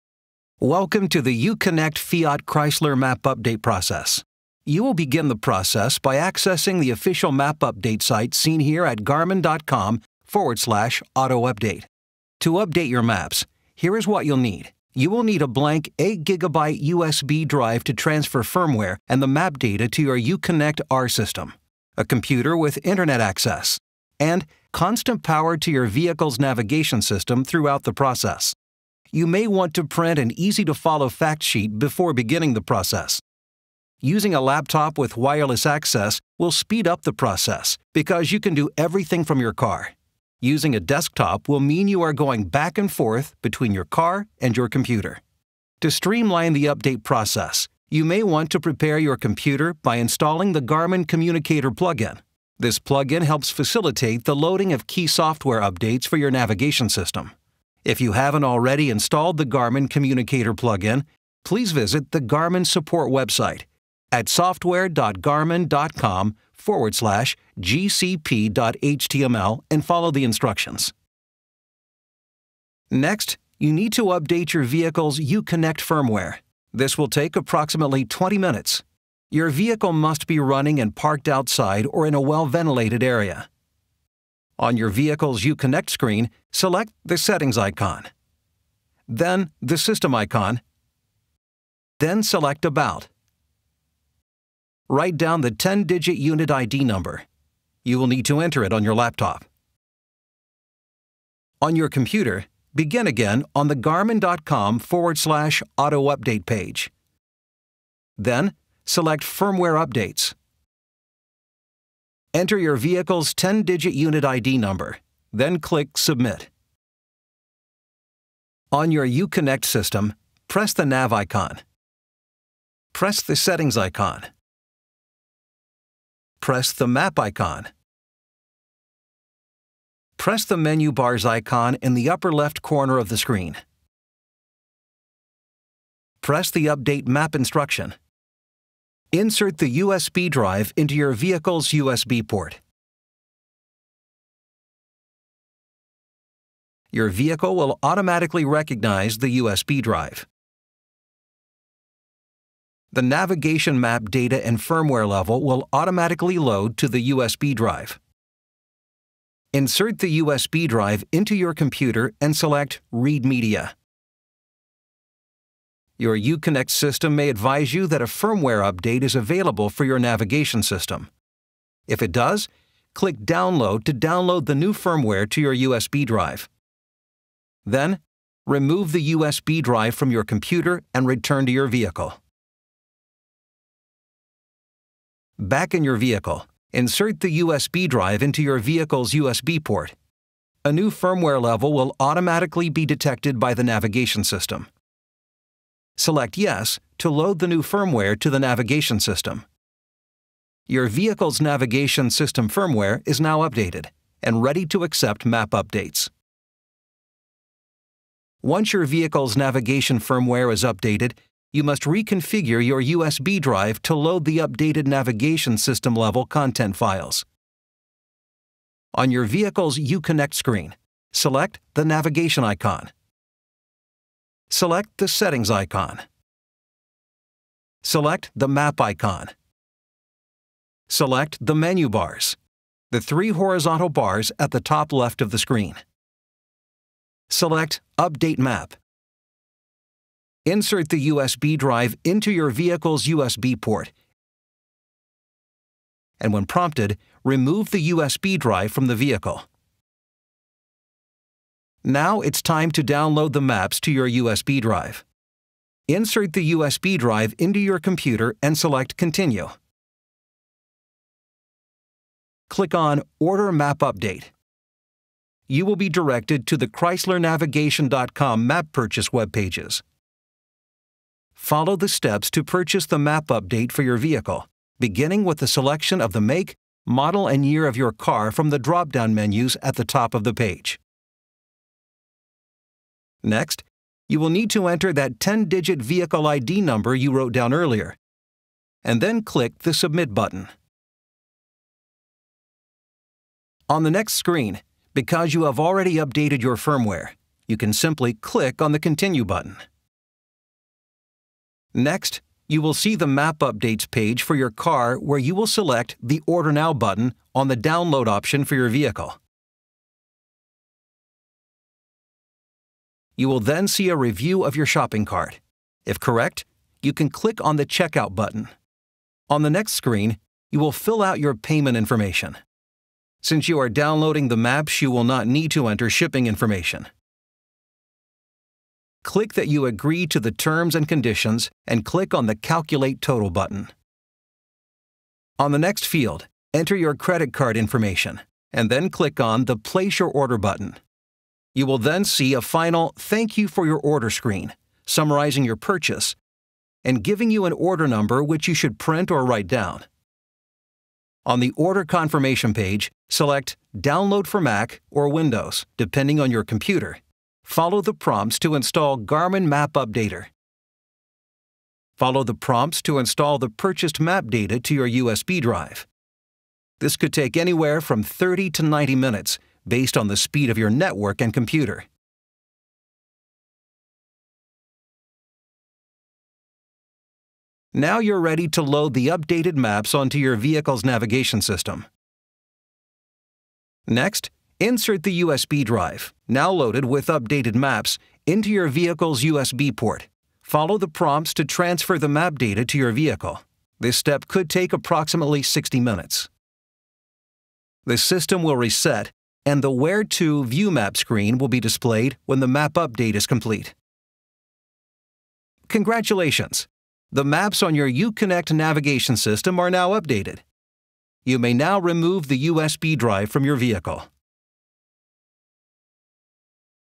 Garmin OEM: Updating Your Maps on Your Fiat Chrysler
Updating your maps on your Fiat Chrysler is as easy as logging onto Garmin.com/auto-update. To ensure success, make sure you have a blank 8 GB USB drive to transfer firmware and map data to our uConnect system before you begin. You also will need a computer, preferably a laptop, with Internet access; constant power to your vehicle’s navigation system; Garmin Communicator Plugin, a free program from Garmin; and your car parked outside, in a well ventilated area. Once you have and/or complete these items and log into the website, you’ll be able to follow our step by step instructions on how to update your in-dash navigator. Plan on it taking more than an hour to complete the process.